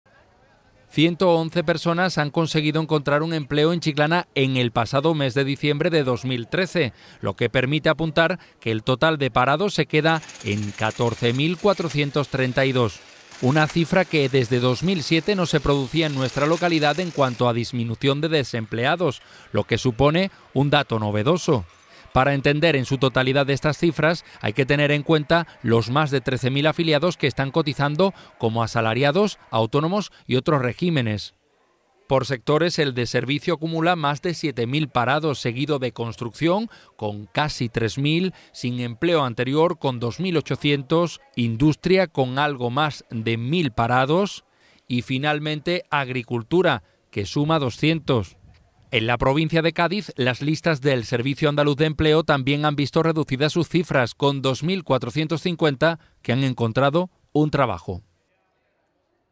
Desciende el paro
Baja el paro en Chiclana en más de cien personas y el alcalde muestra su satisfacción aunque reconoce que no se puede ser triunfalista porque todavía quedan muchos ciudadanos sin tener un trabajo. Cree sin embargo, que se va por el buen camino.